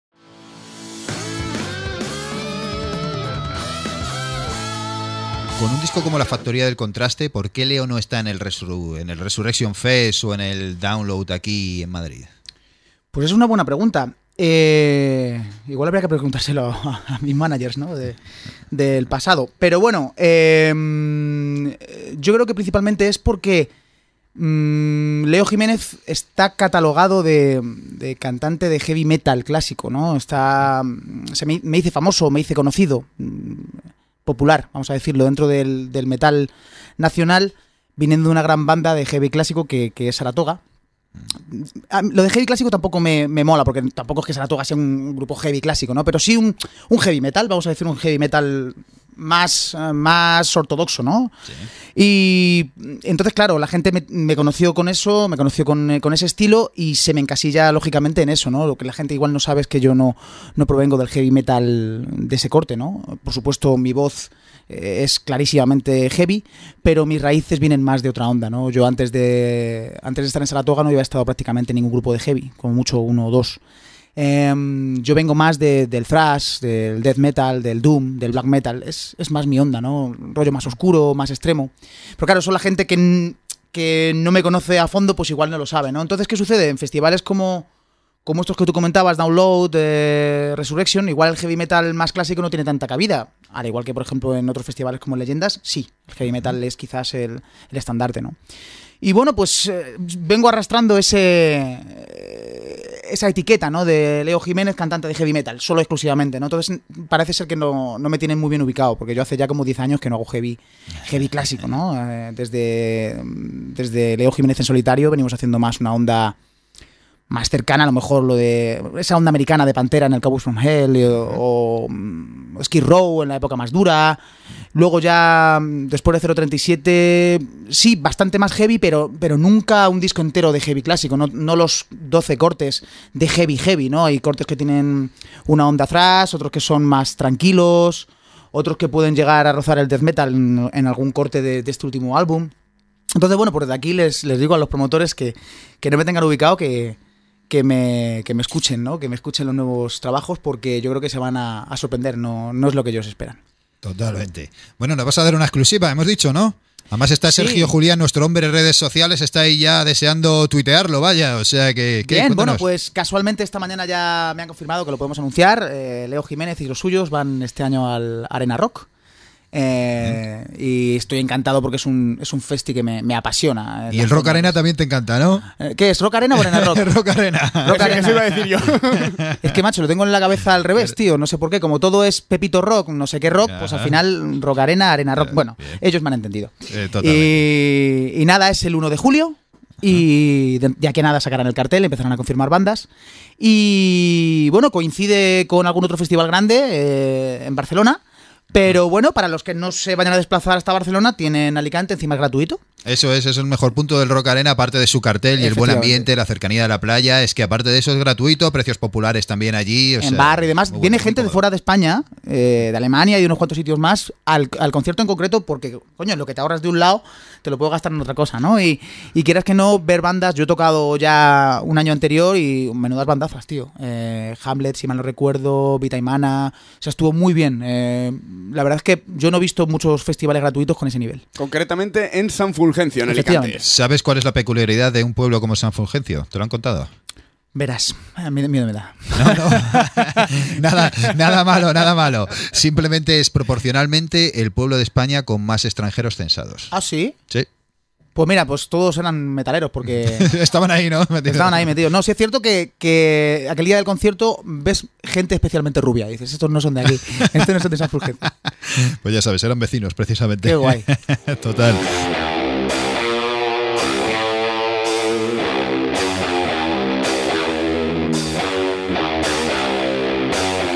¿Por qué Leo Jiménez no está en el Resurrection Fest ni en el Download?
La Bestia aclara por qué no está en ninguno de estos dos conocidos festivales... pero confirma en cual sí que tocará este próximo verano... ¡No te lo pierdas!

Vídeo y Edición: Sara Riesgo.